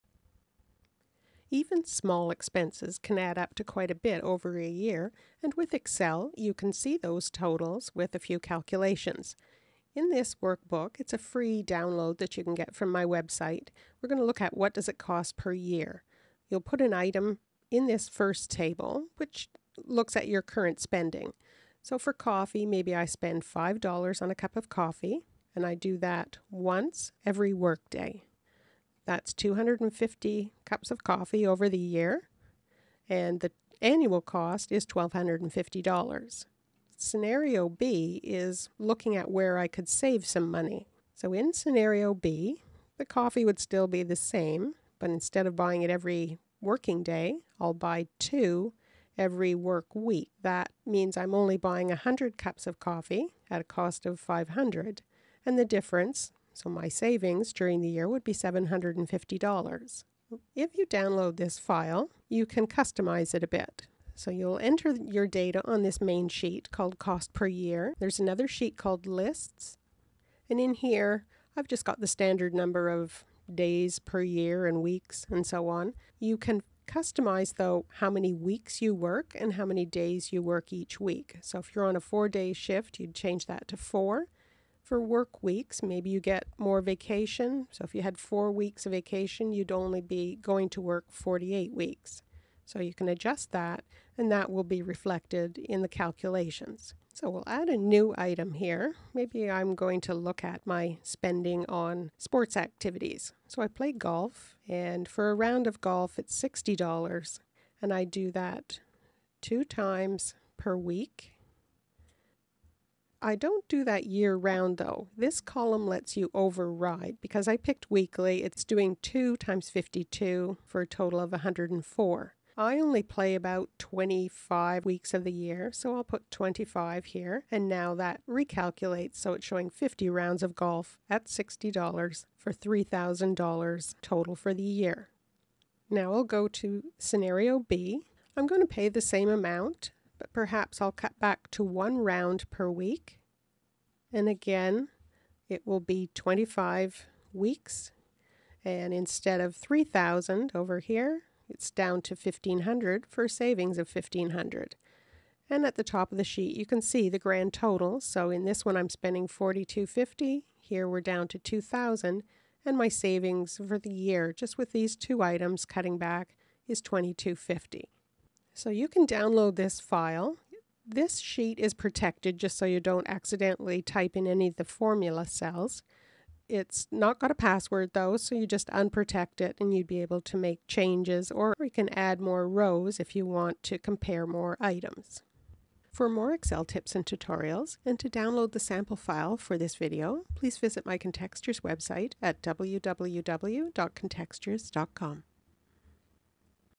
Calculate Annual Costs and Savings with Excel Template
Small recurring expenses can add up to a large amount over a year, and you can use this Excel template to see the annual totals.

Enter your current expenses in Scenario A, and enter your potential cost reductions in Scenario B.

Formulas calculate the annual totals for both scenarios and show your the item differences, and the total savings.